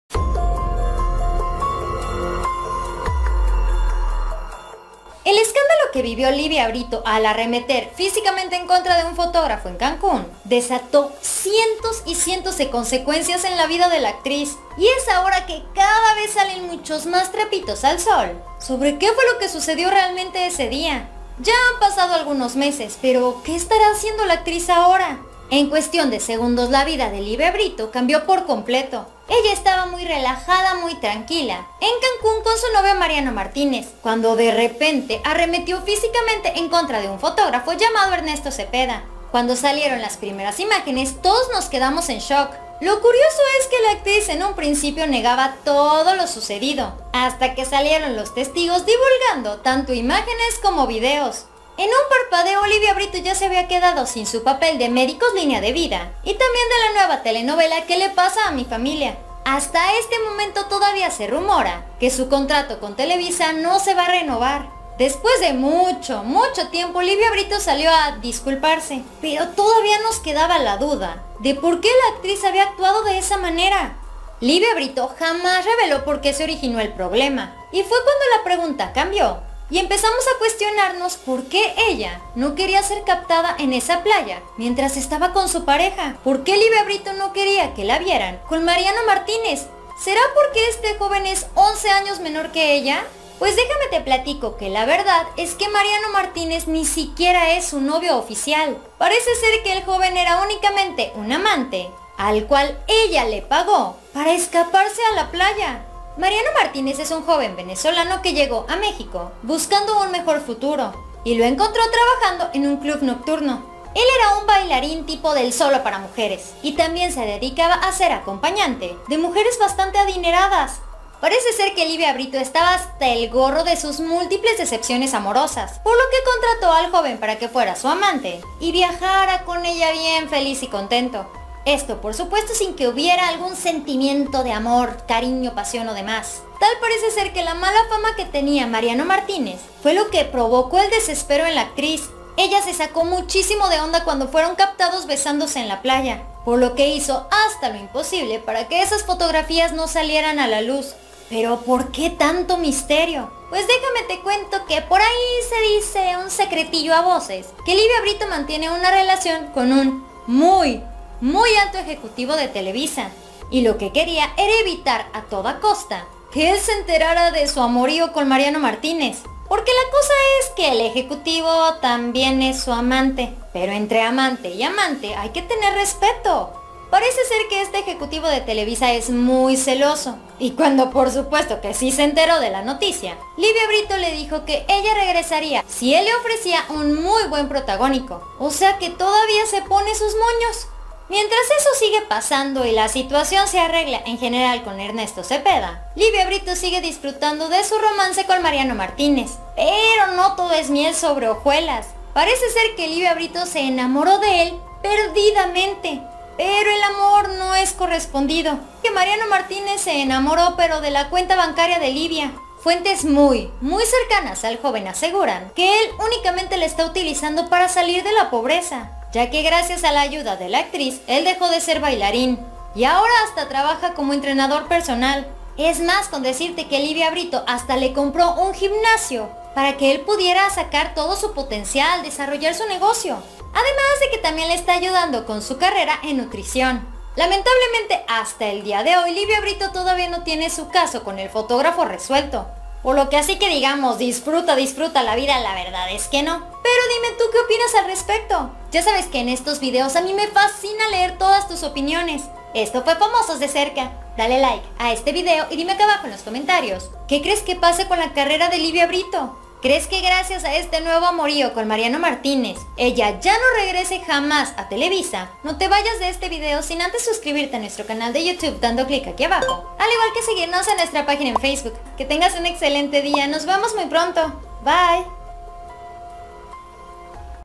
🚨😱 ¡ 𝗦𝗮𝗹𝗲 𝗮 𝗟𝘂𝘇 𝗧𝗼𝗱𝗼 𝗦𝗼𝗯𝗿𝗲 𝗟𝗶𝘃𝗶𝗮 𝗕𝗿𝗶𝘁𝗼 No lo Podras Creer ! ⛔💥
Se revela la razón porque no queria que tomaran fotografias.

 BIENVENIDOS!!!
Famosos
Escandalos
Escandalo de los famosos
Farandula
Televisa
Telemundo
Tv azteca